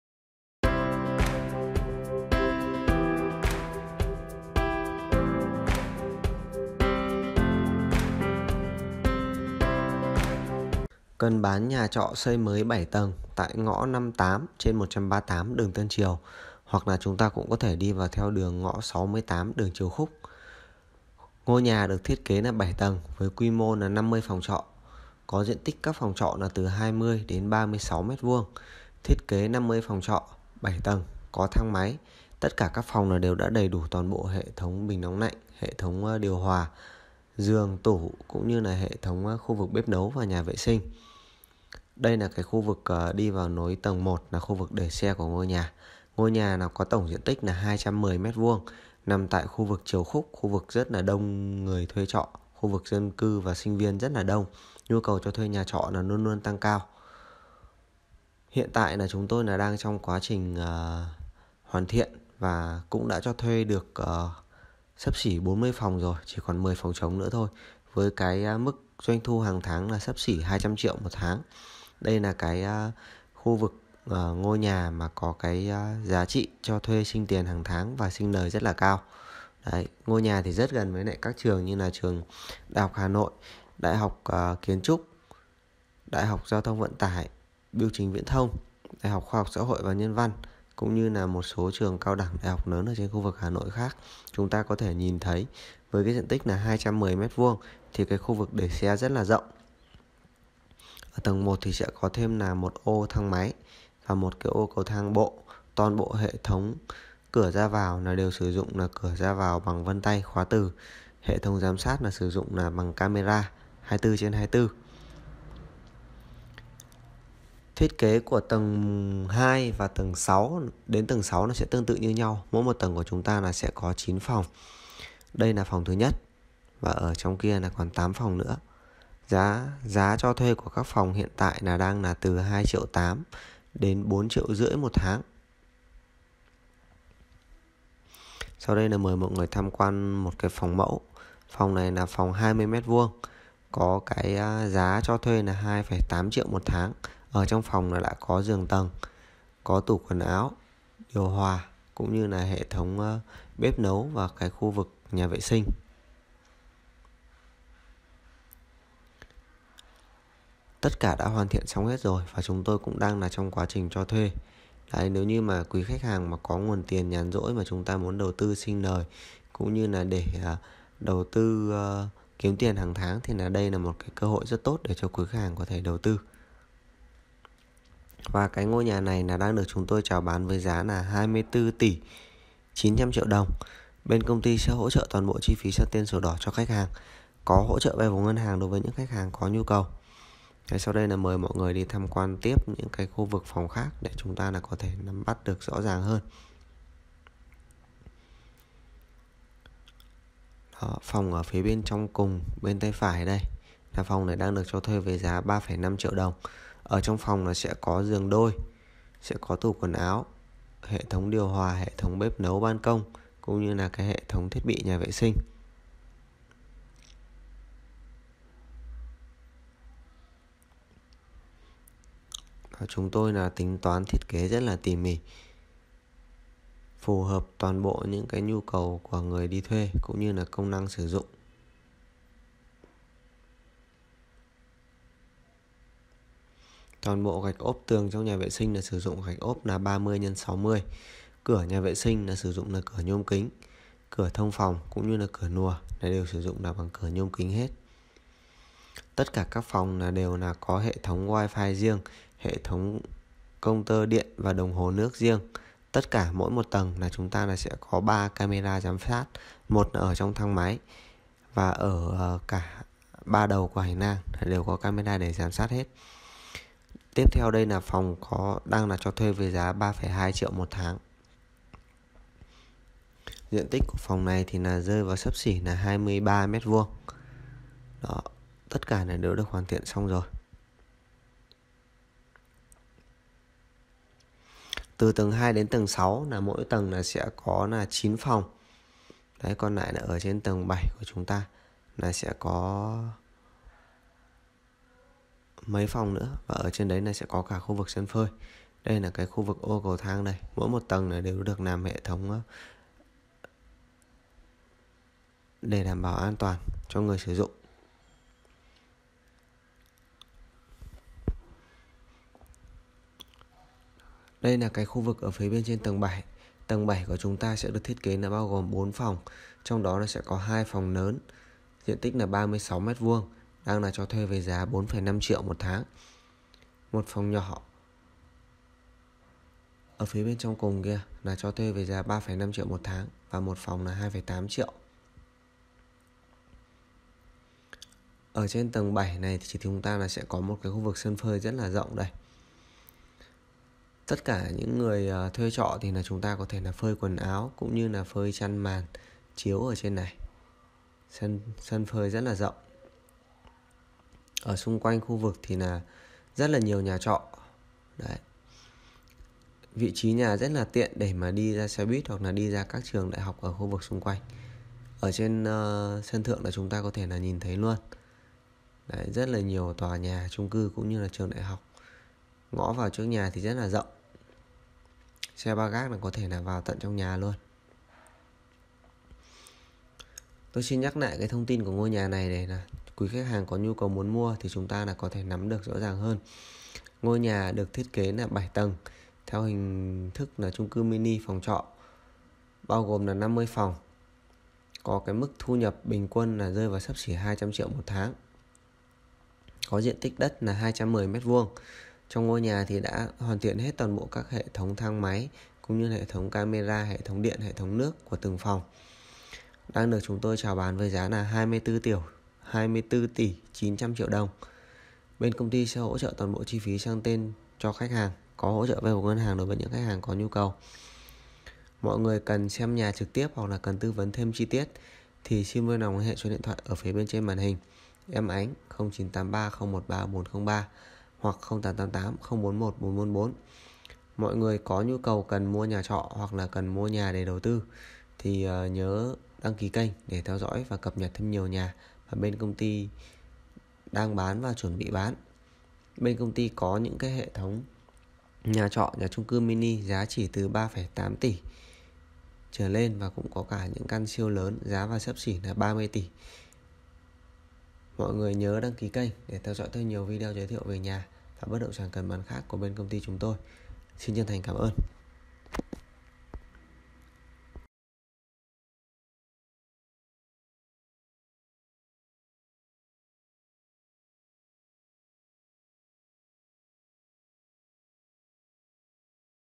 Bán Nhà Trọ Triều Khúc 50 Phòng, 7 Tầng Thu Nhập Hơn 200 Triệu 1 Tháng | nhà TỐT
Bán Nhà Trọ Triều Khúc 50 Phòng, 7 Tầng Thu Nhập Hơn 200 Triệu 1 Tháng
- Địa chỉ: số 58/138 Tân Triều hoặc đi vào ngõ 68 Triều Khúc
- Diện tích: 210m2
- Hướng nhà: Tây Nam
- Thiết kế: 7 Tầng, 50 phòng khép kín có đủ: điều hòa, nóng lạnh, giường tủ, khu bếp nấu, TBVS...